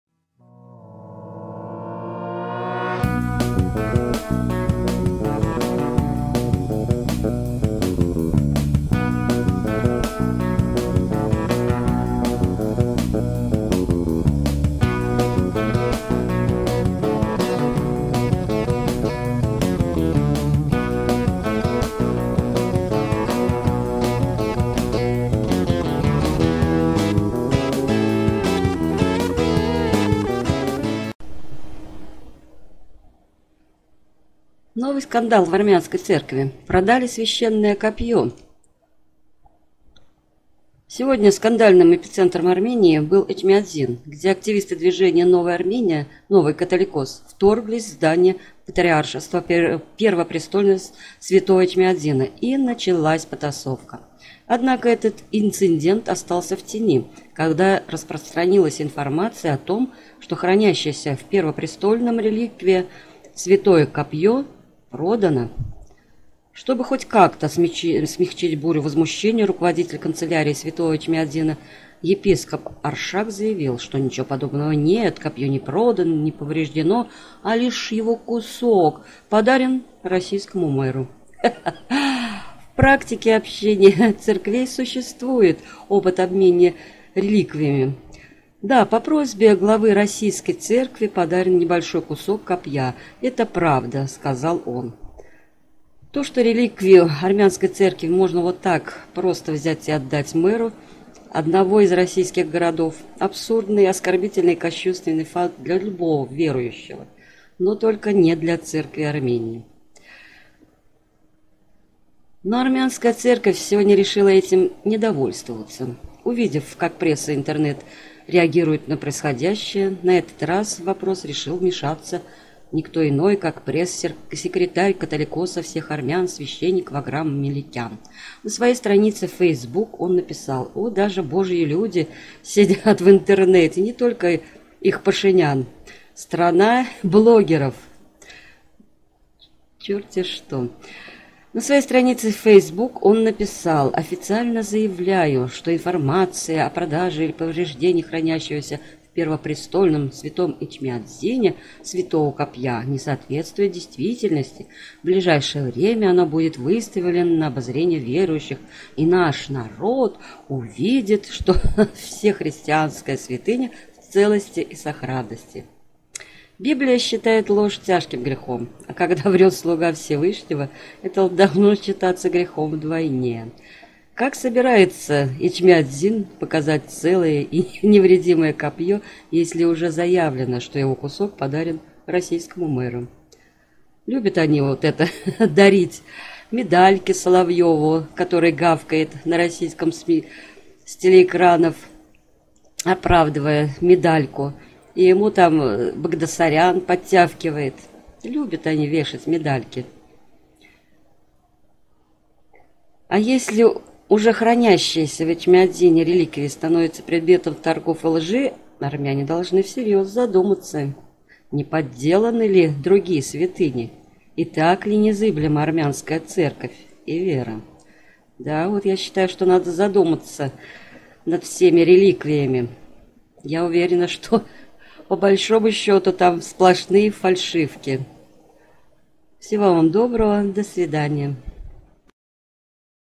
Новый скандал в армянской церкви: продали священное копье
Библия считает ложь тяжким грехом. А когда врет слуга Всевышнего, это должно считаться грехом вдвойне. Как собирается Эчмиадзин показать целое и невредимое копье, если уже заявлено, что его кусок подарен российскому мэру? А если уже хранящиеся в Эчмиадзине реликвии становятся предметом торгов и лжи, армяне должны всерьез задуматься, не подделаны ли другие святыни, и так ли незыблема армянская церковь и ...вера.